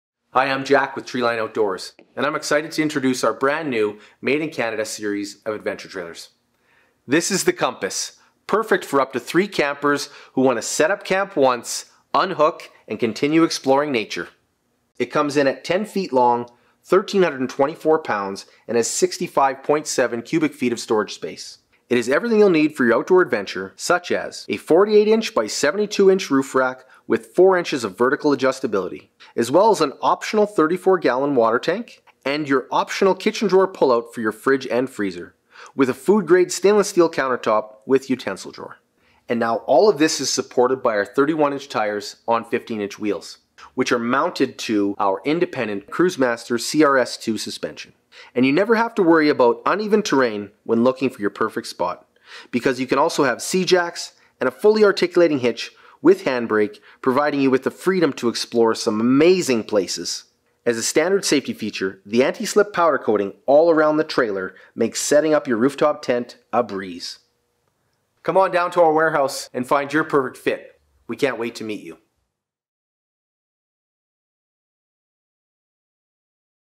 The Compass Adventure Trailer
Maximize your experience outdoors with The Compass Adventure Trailer. Compact, highly maneuverable, and capable of storing everything you need to make your camping experience easy, fun, and memorable.